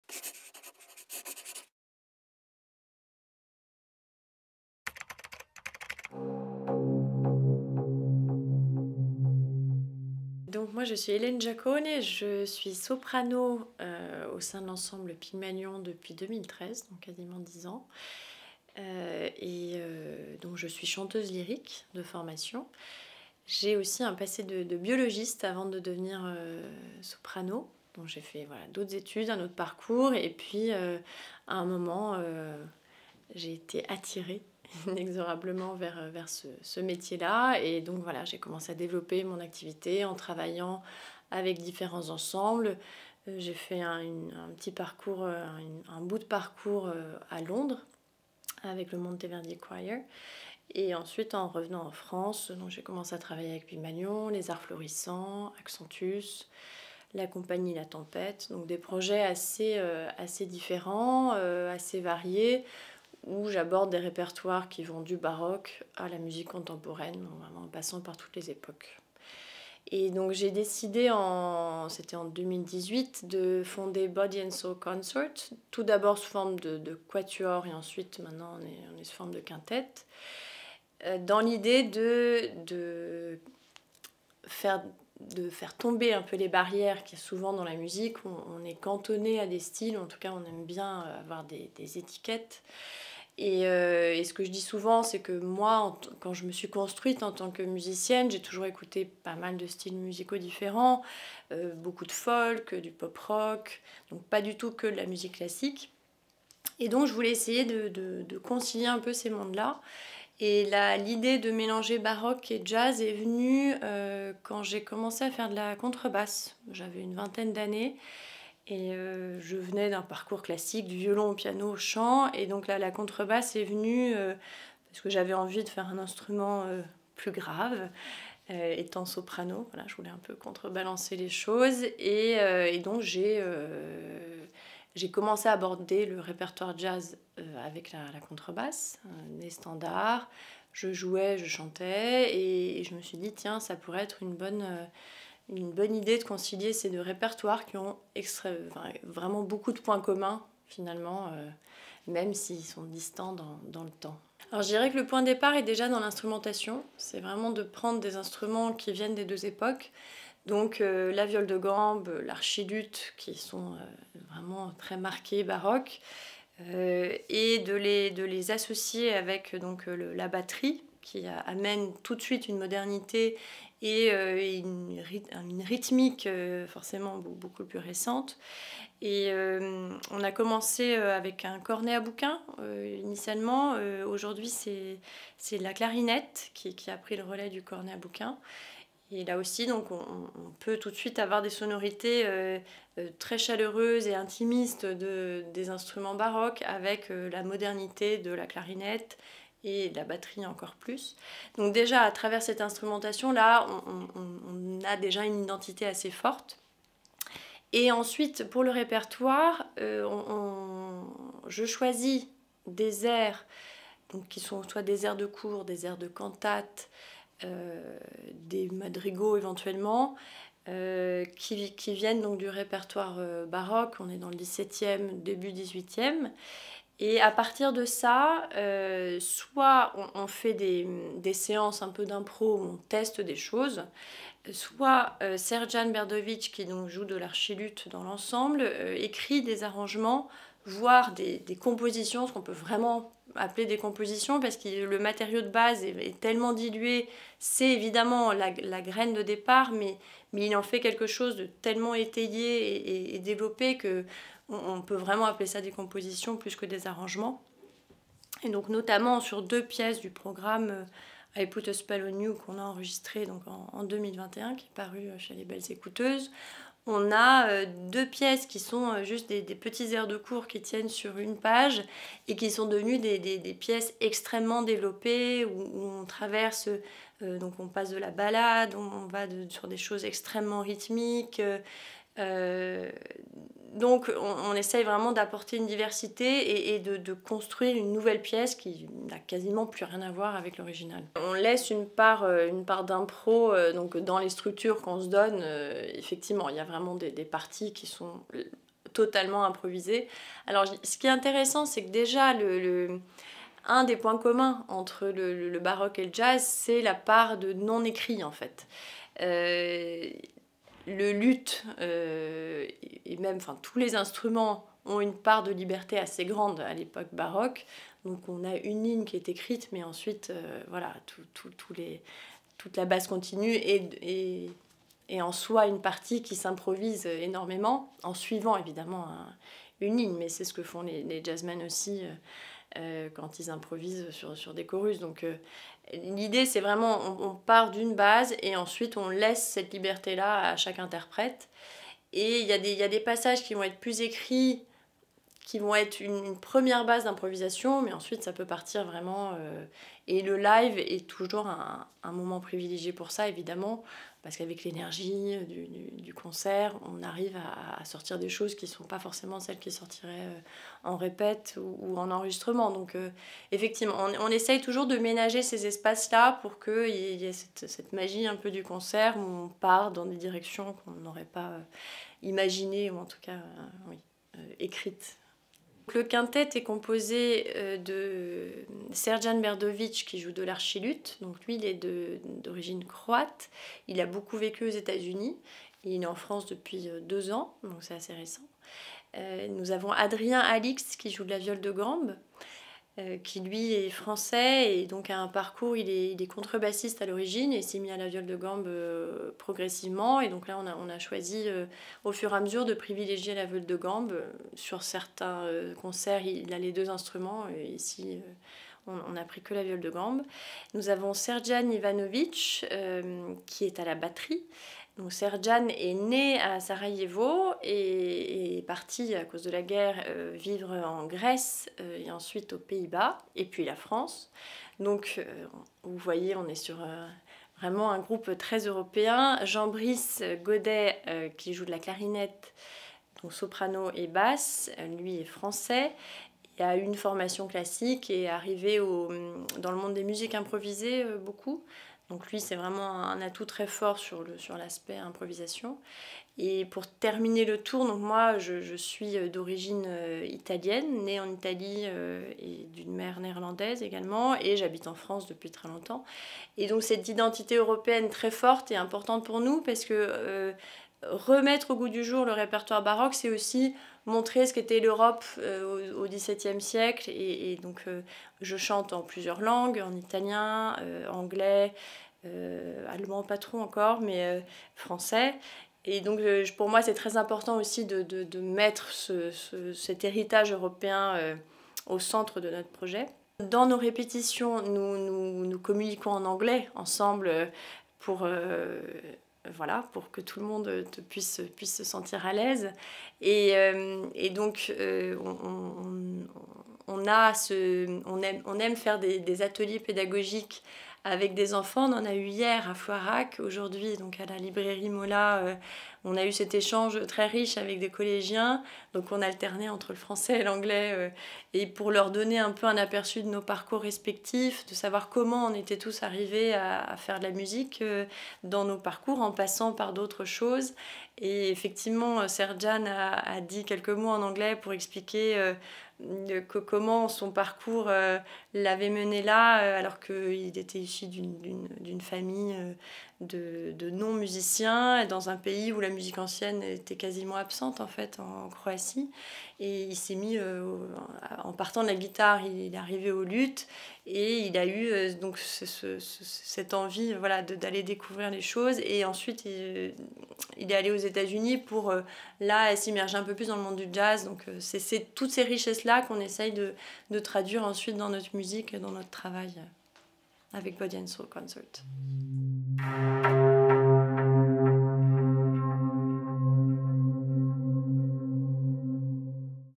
Quintette Body & Soul Consort - Ellen Giacone
Le quintette Body & Soul Consort associe deux langages musicaux distants de plus de trois siècles. Le baroque de l’ancien monde et le jazz du nouveau monde dialoguent avec passion et subtilité.La voix de la soprano italo-néerlandaise Ellen Giacone donne le ton. Avec cinq musiciens, ils font résonner ou swinguer viole de gambe, archiluth, cornet à bouquin, mais aussi batterie et basse électrique. En partenariat avec Pygmalion.